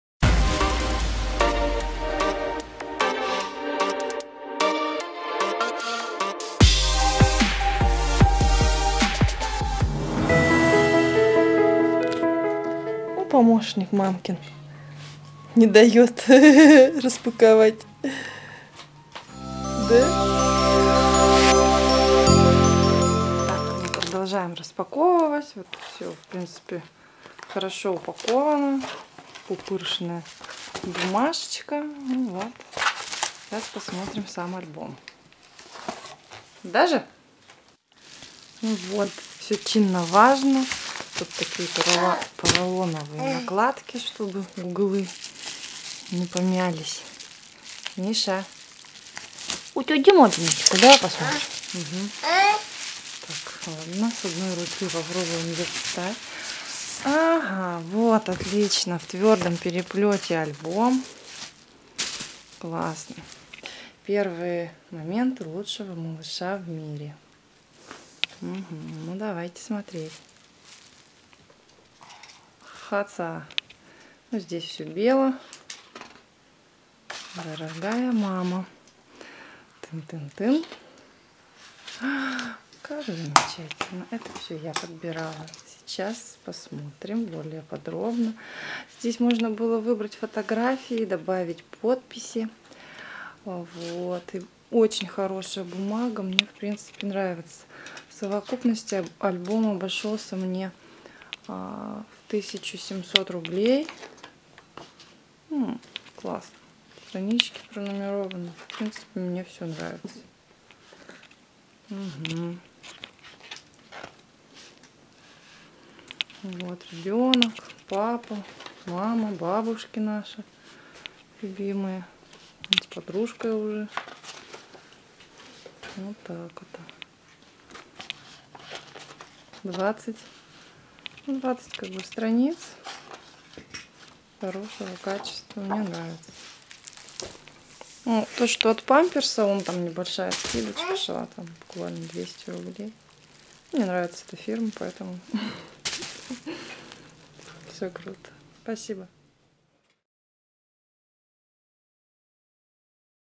Детский альбом на годик
В качестве подарка ребёнку на милан, заказала фотоальбом. Не экономьте на качестве. Выбирайте твёрды переплёт и хорошую бумагу. Ведь альбом останется с вами на всю жизнь!